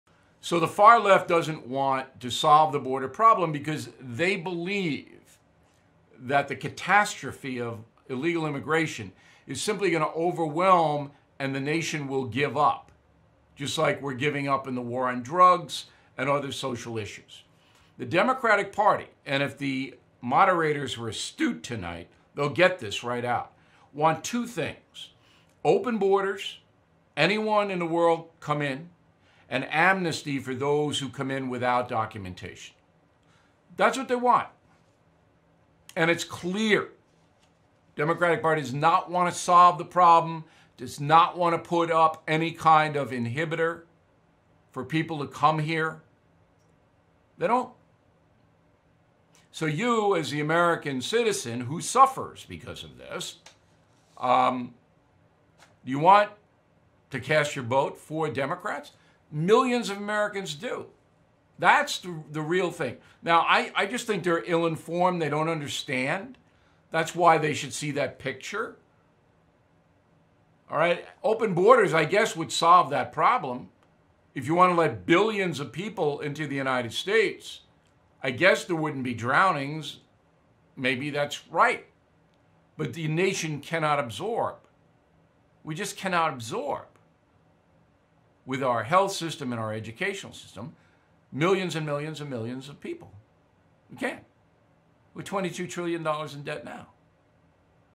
The Far-Left Doesn't Want to Solve the Border Problem
As many on the left have claimed that the crisis at the border has been manufactured by the Trump administration, there is clear evidence that there is indeed a crisis. Congress has failed to act and our system is overwhelmed-the nation cannot absorb everyone that shows up at our borders.